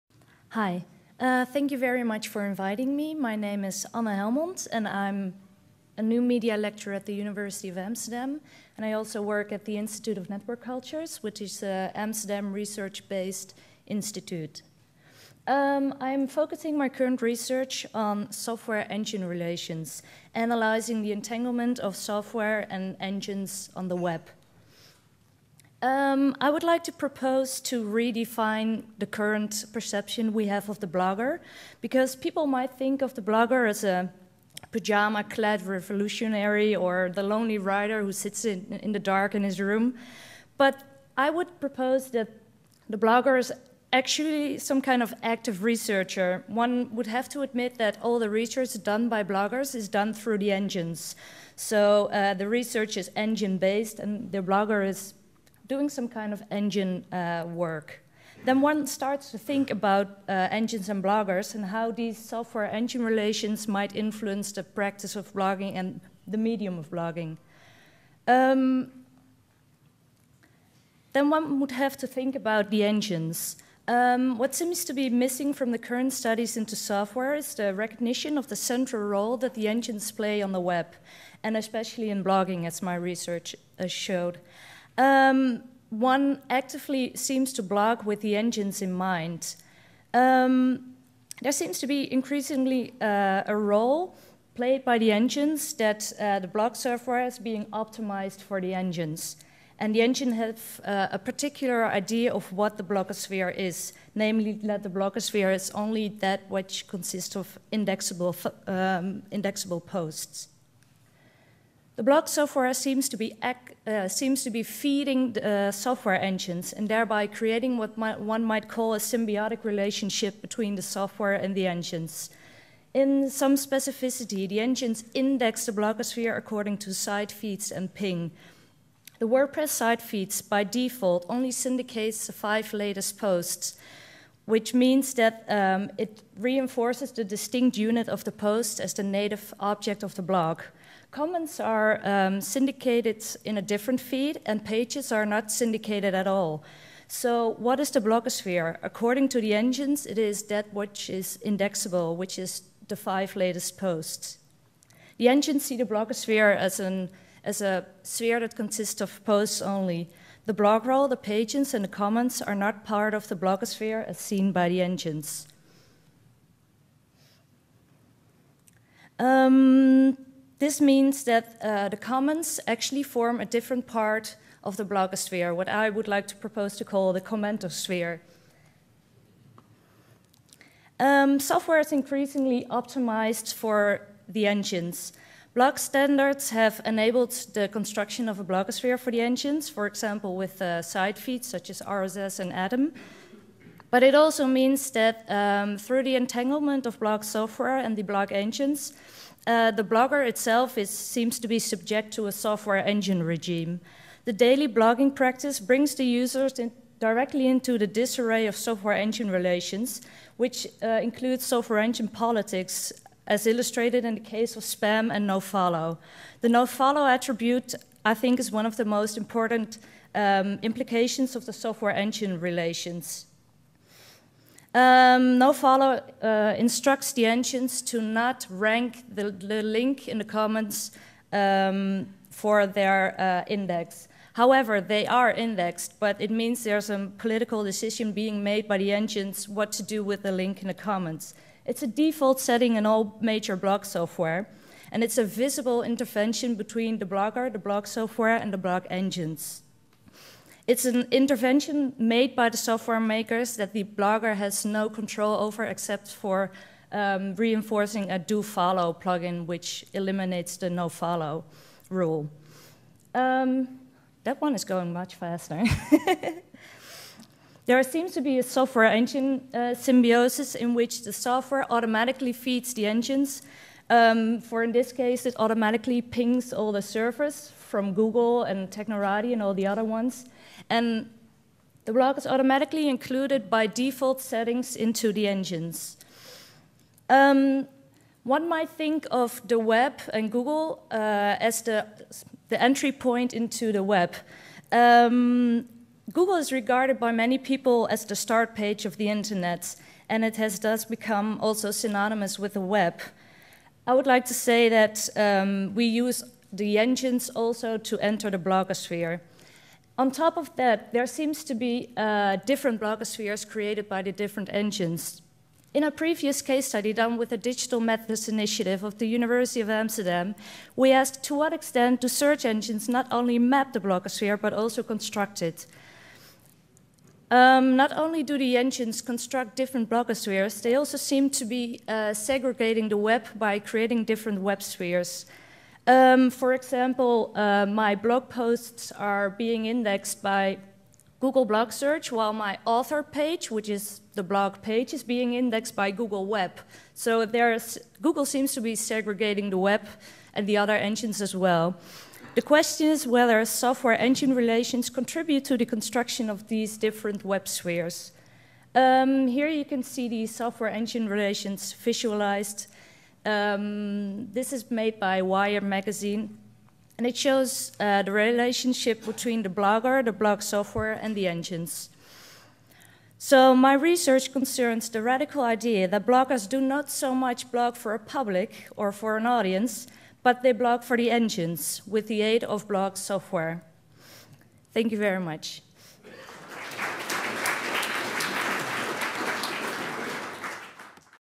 SoftWhere'08 (#06): Anne Helmond
"Software-Engine Relations" by Anne Helmond.

SoftWhere 2008 was a foundational event bringing together key U.S. figures in the emerging area of Software Studies. The workshop discussed the meaning of studying software cultures, and the direction and goals of Software Studies as an emerging movement.